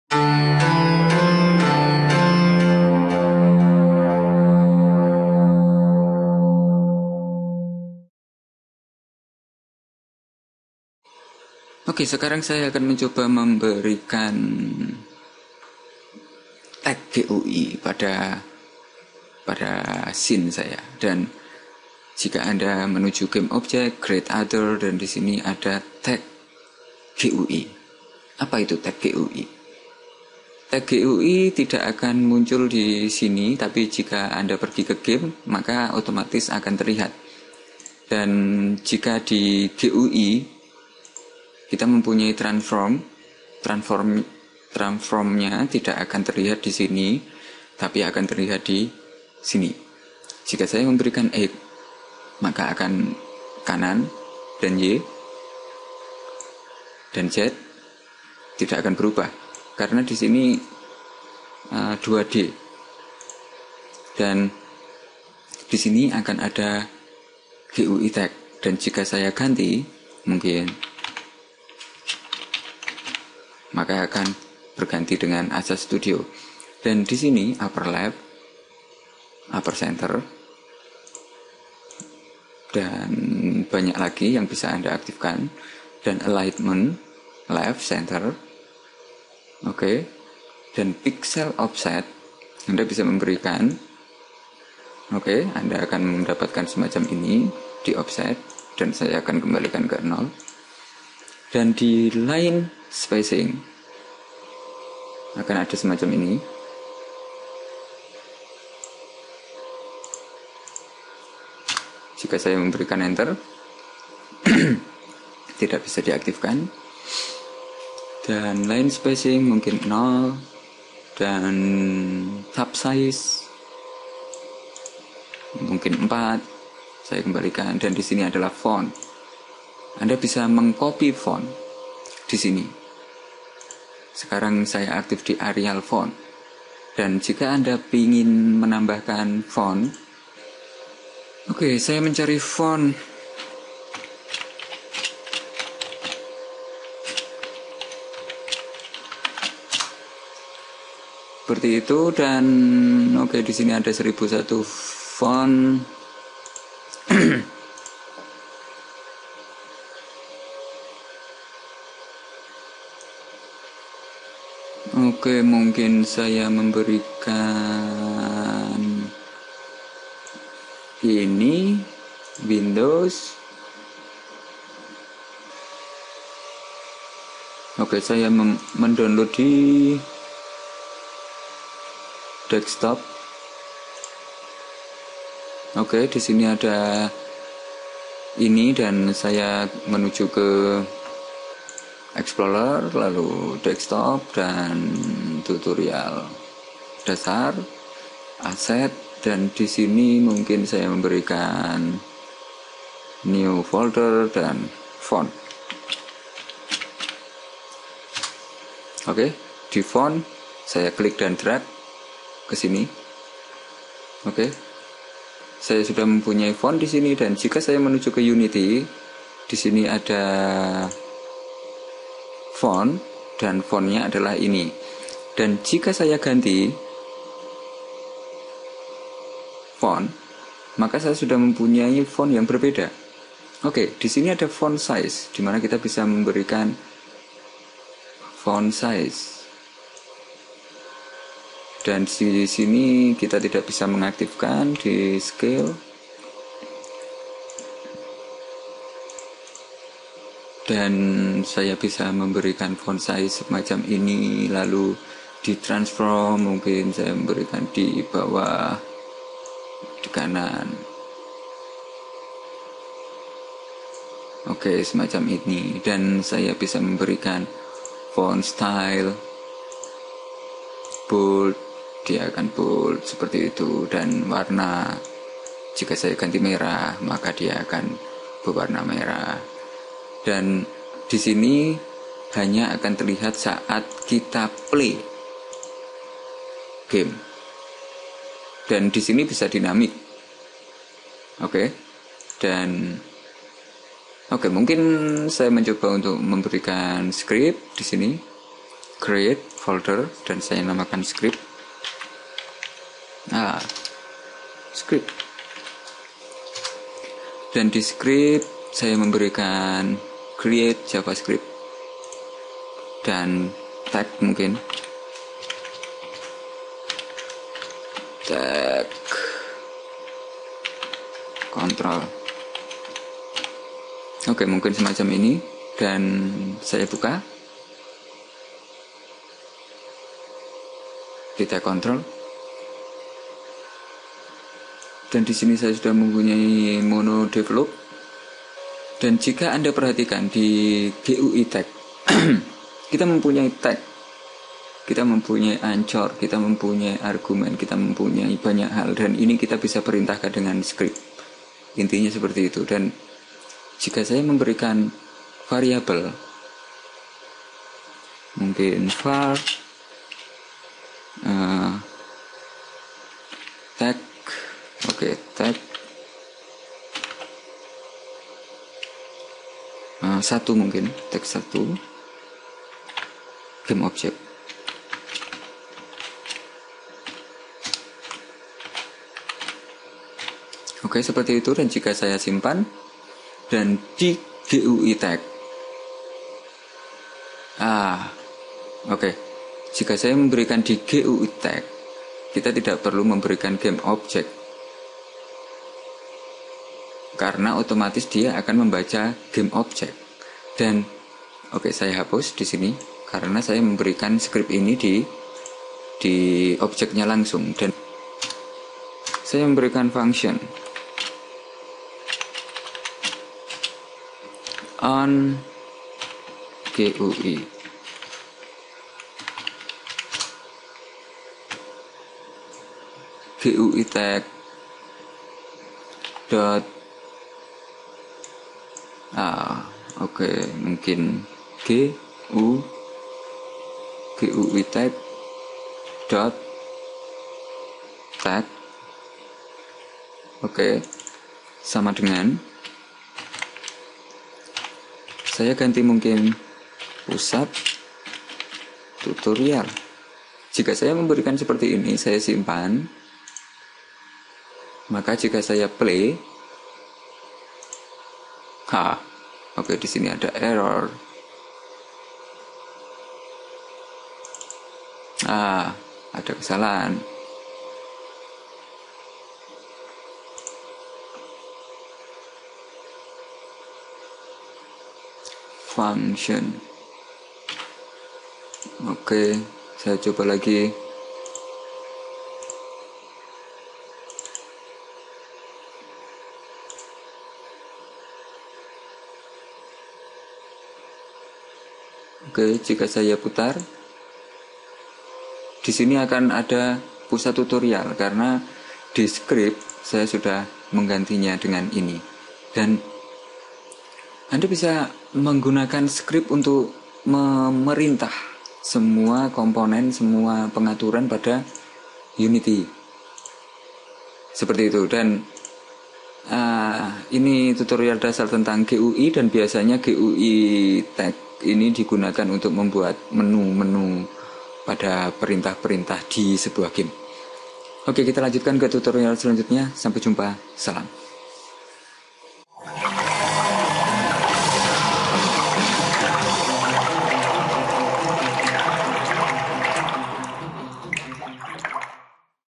Unity3d Tutorial Dasar [10] GUI Text
Di video ini terdapat penjelasan tentang cara mendownload dan menginstal Unity3d untuk window.

Semoga bermanfaat.
Salam